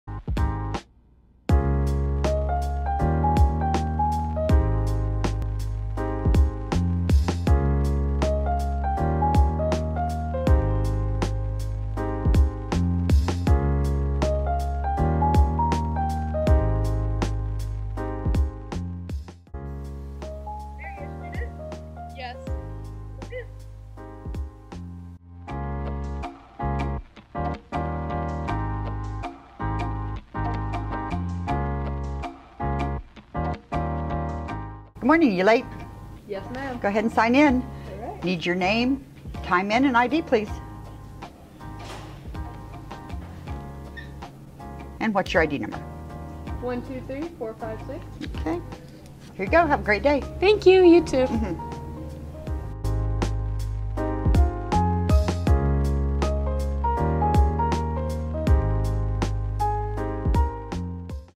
Tardy Arrival Procedure
Oh no! Are you late to school? Here's the proper procedure for how to check in.